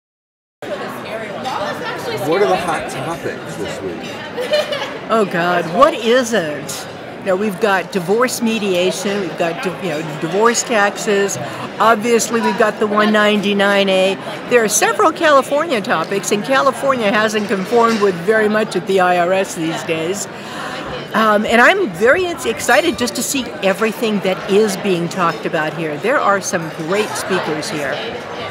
"Tax Mama" Eva Rosenberg, EA: Hot Topics at CSTC$ 2019
"Tax Mama" Eva Rosenberg, EA, previews the California Society of Tax Consultants' 33rd annual Summer Symposium at the Westgate Las Vegas Resort & Casino in Las Vegas, June 9-12.